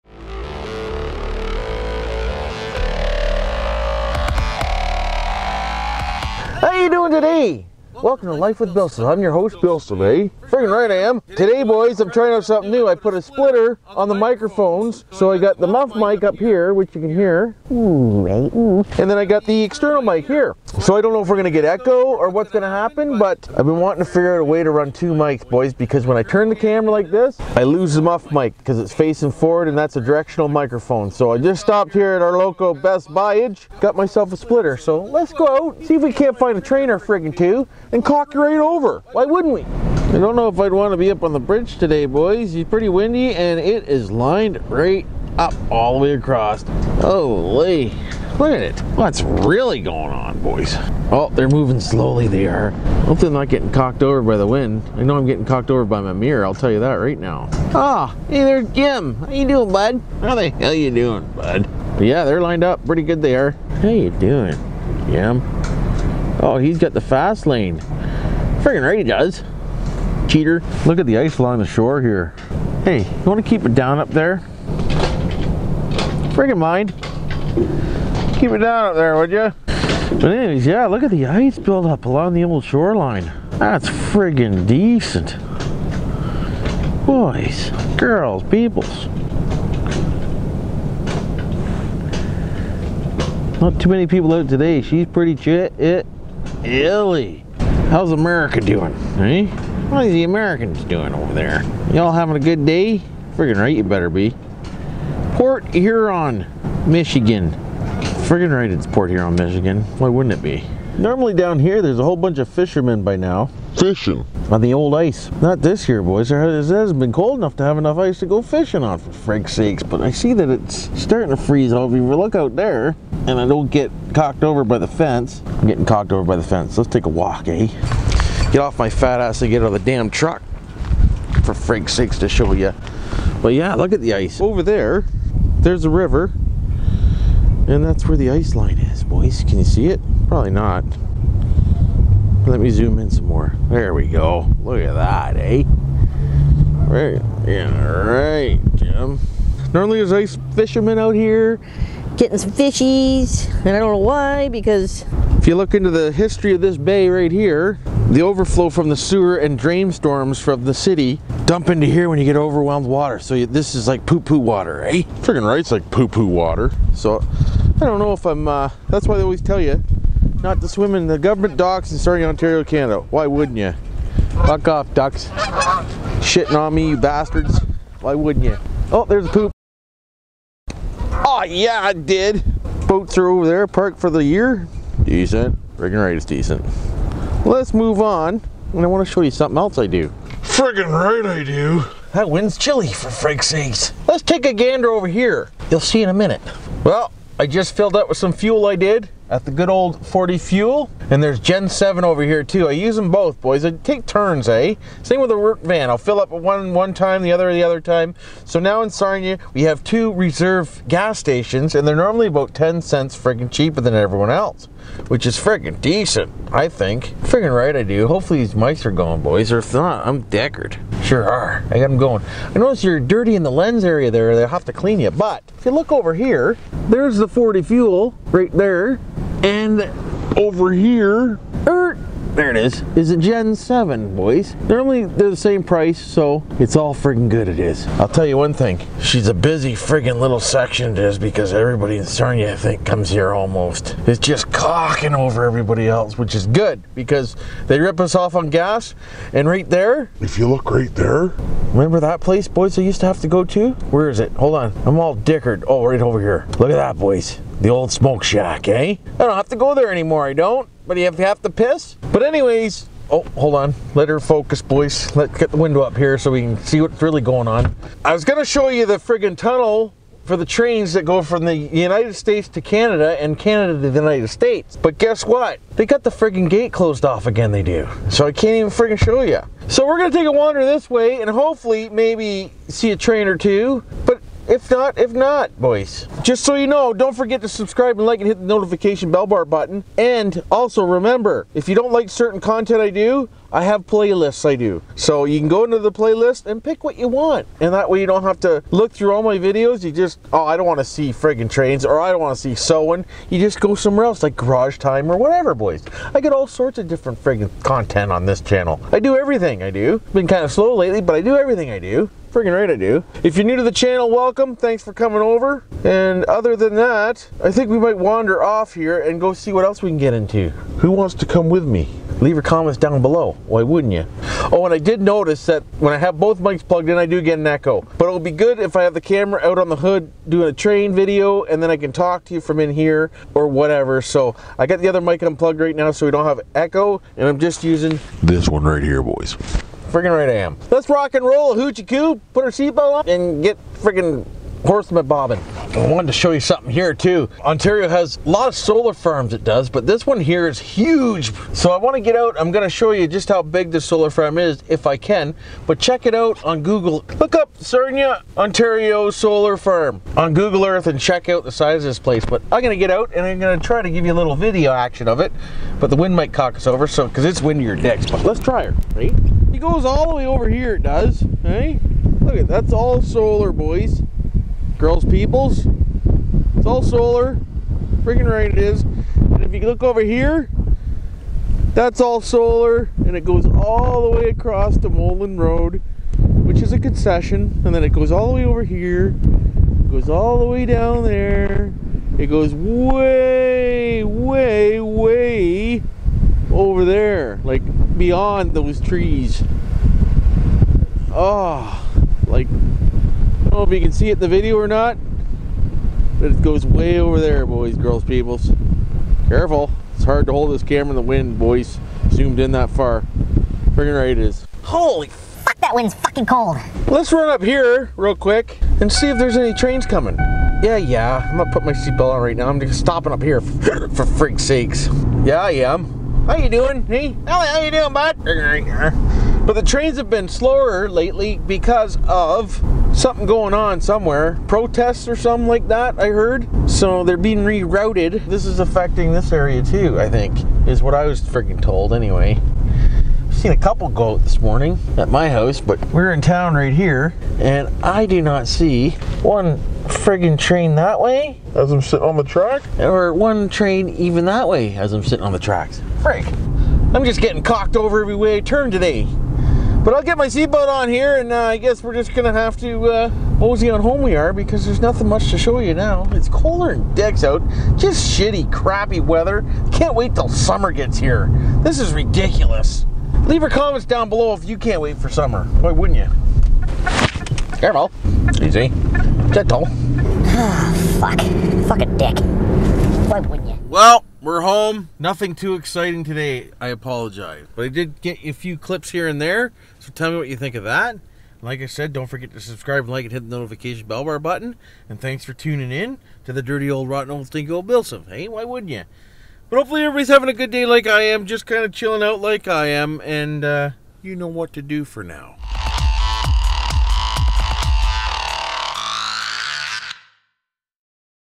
Let's Get Some Air
Had to get out for a drive even know it was not sunny or warn but starting to go house crazy over here

BillSiff (GoGlass)
1289 Confederation St,
Unit 3
Sarnia Ont.
Canada
N7S 4M7

2540 Lapeer Rd,
Port Huron,